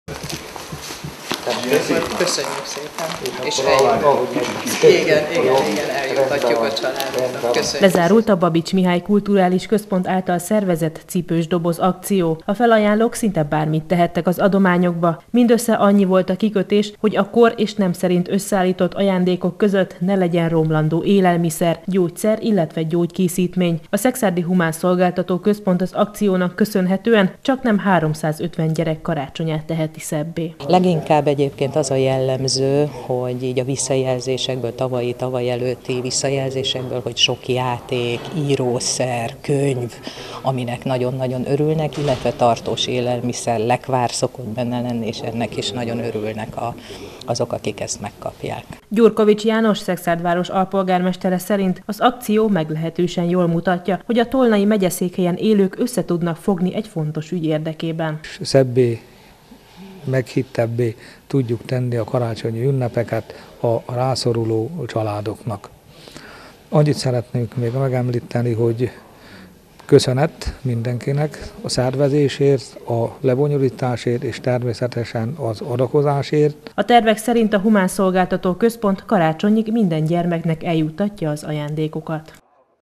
Cipősdoboz akció - közel 350 gyerek karácsonyát teszik szebbé idén Szekszárdon
Idén csaknem 350 ajándékkal teli cipősdobozt adományoztak a szekszárdiak a helyi Humánszolgáltató Központ által pártfogolt gyerekeknek. A Babits Mihály Kulturális Központ ugyanis ismét megszervezte, a már hagyománynak számító Cipősdoboz akciót. A kor és nem szerint összeállított felajánlásokat a tervek szerint még karácsony előtt megkapják a kicsik.